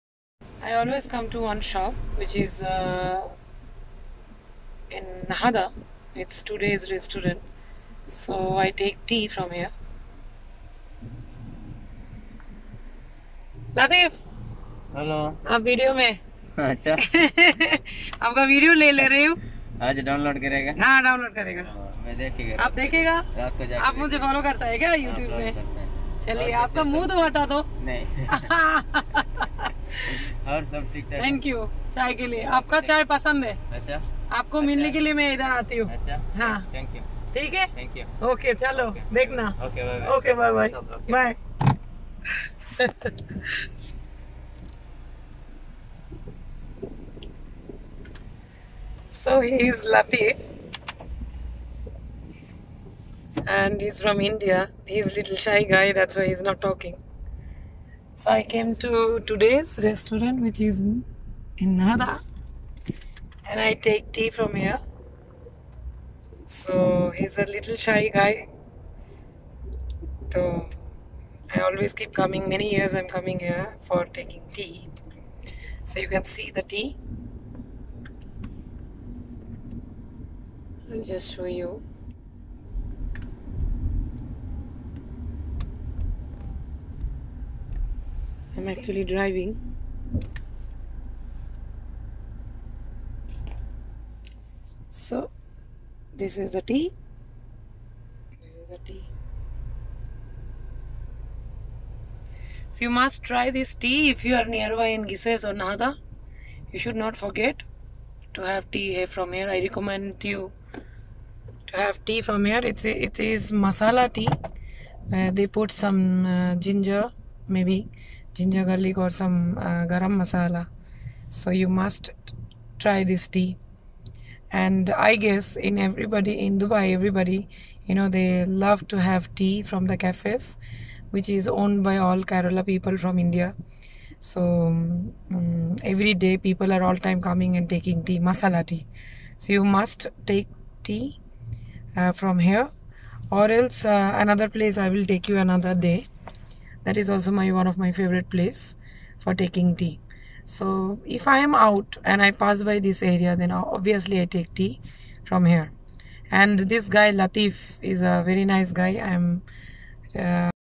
Masala Tea in Dubai | Todays Restaurant | Al Nahda | Mashoor Media | Tahera Vlogs
Tea in Dubai | Todays Restaurant | Al Nahda | Mashoor Media | Tahera Vlogs

Masala tea, Karak Tea, Fresh Milk Tea is famous in Dubai. If you are in Dubai you must try the variety of Tea.
Which tea do you like if you are in dubai. Comment in the comment section 😊😁